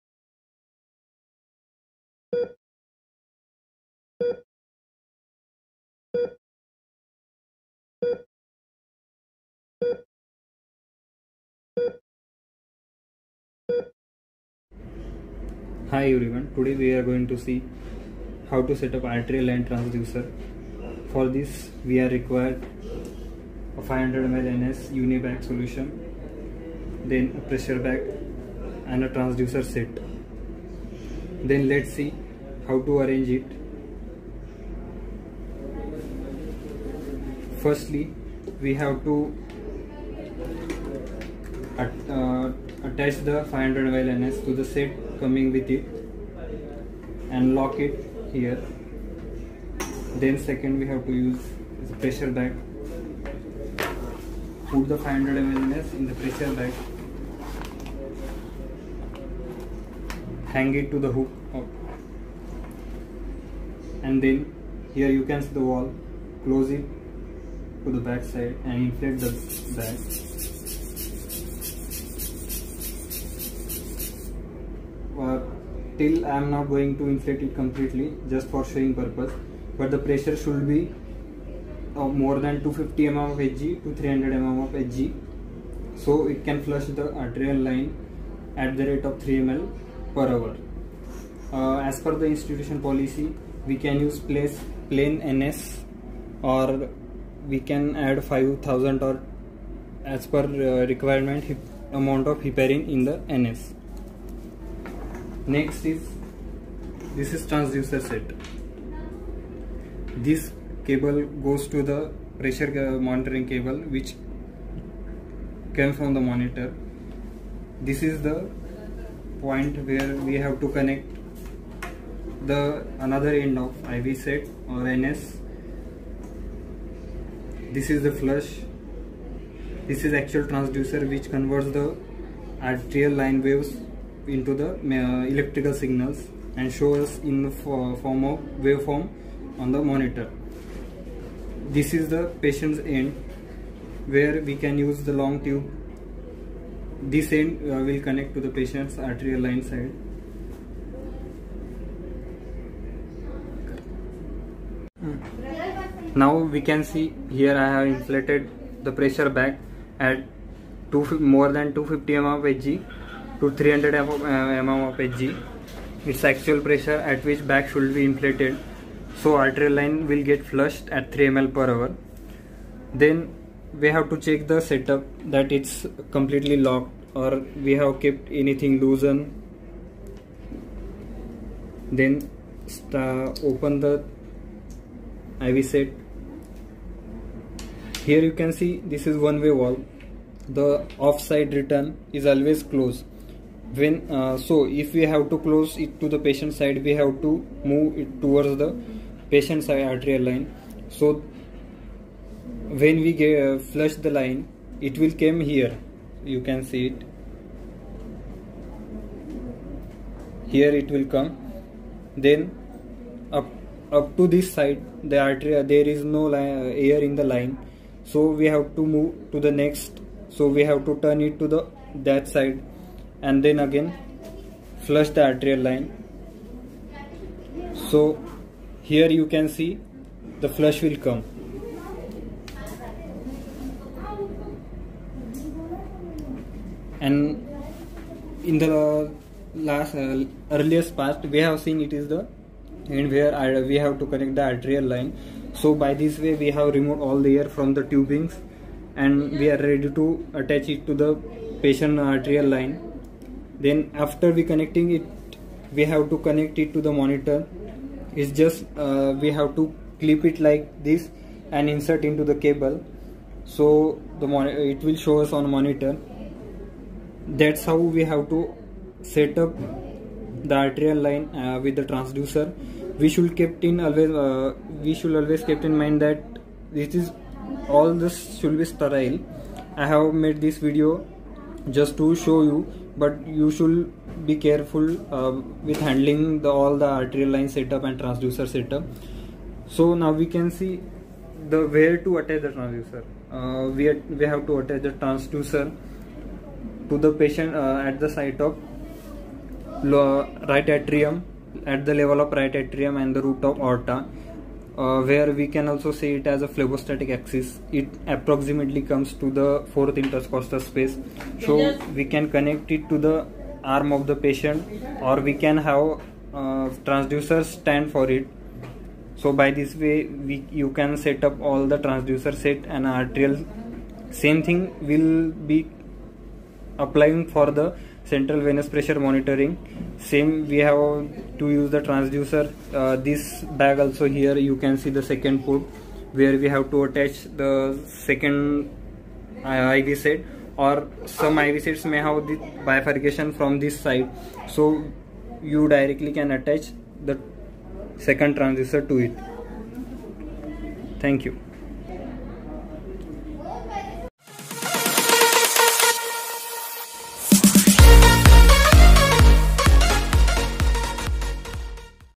Arterial Blood Pressure monitoring + Transducer setup + Pressure bag setup demo.
For showing purpose I have made this video. Always keep in mind that Arterial line transducer setup, flush is an sterile procedure. Handle it carefully.
 For any doubt comment 👍.